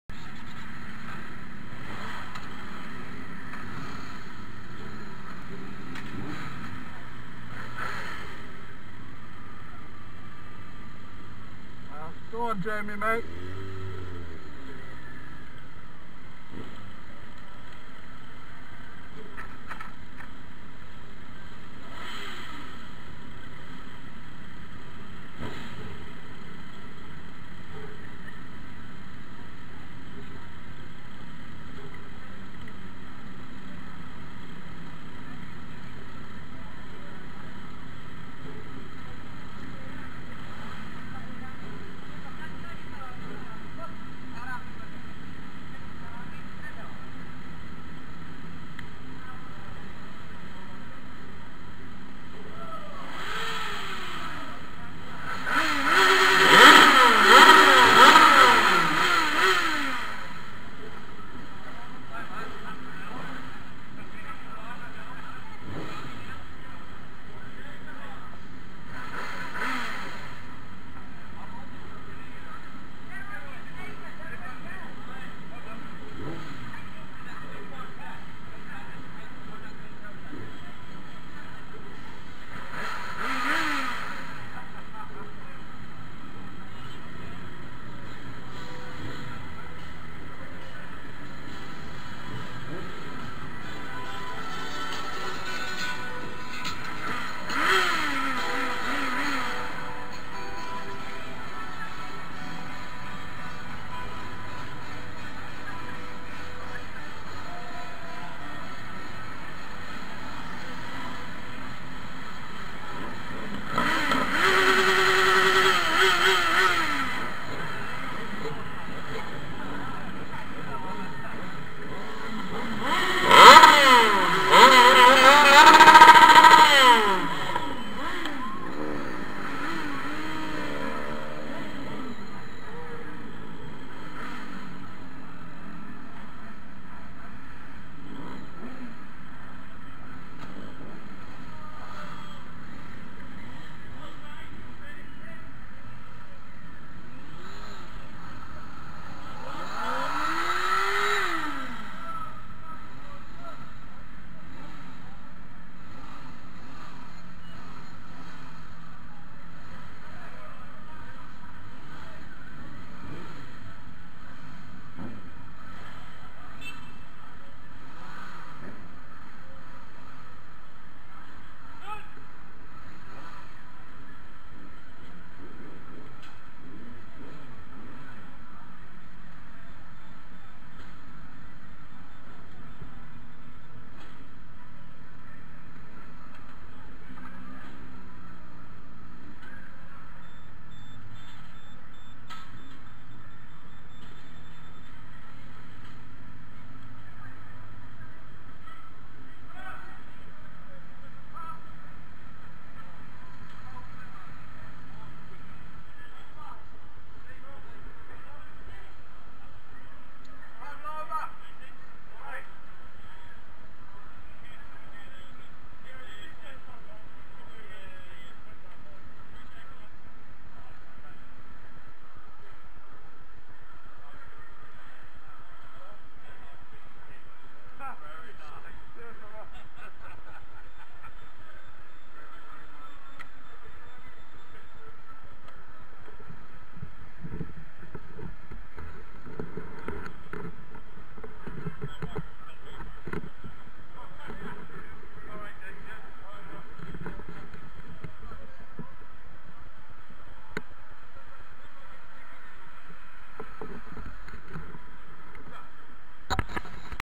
German Market ride
We Went to the German Market on friday night